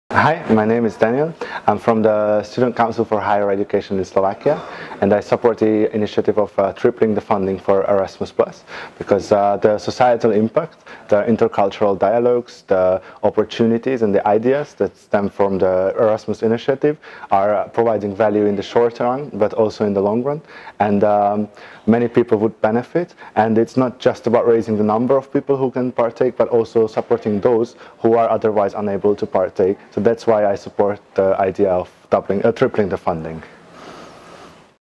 Invest in our Future! Erasmus3x - Daniel Csefalvay
Students in Europe unite behind one clear demand: Invest in our future, invest in Erasmus+!

We urge decision makers to triple the funds for the next cycle of the ERASMUS+ programme. Continued investment in Erasmus+ is needed in order to reach the European wide target of 20% mobile students.